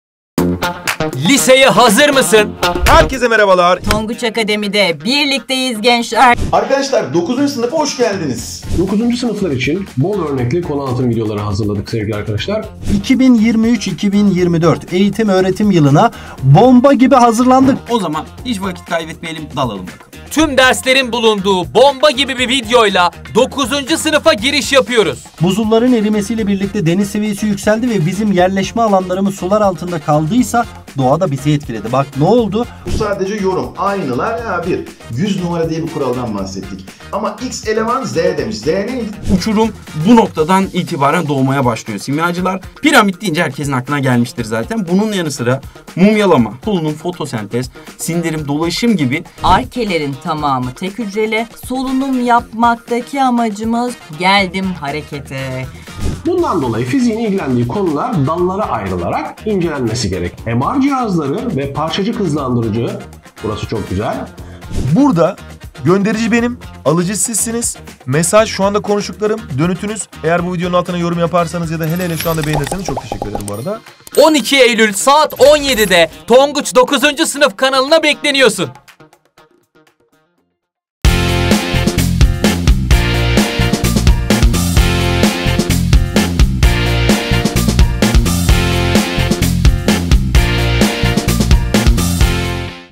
Liseye Hazır Mısın❓ 9. SINIF DERSLERİ 12 EYLÜL'DE BAŞLIYOR❗
9.Sınıfa geçtik, artık liseli olduk! tonguç'ta tam gaz derslere devam ediyor, seni 12 Eylül'de tonguç kanala bekliyoruz!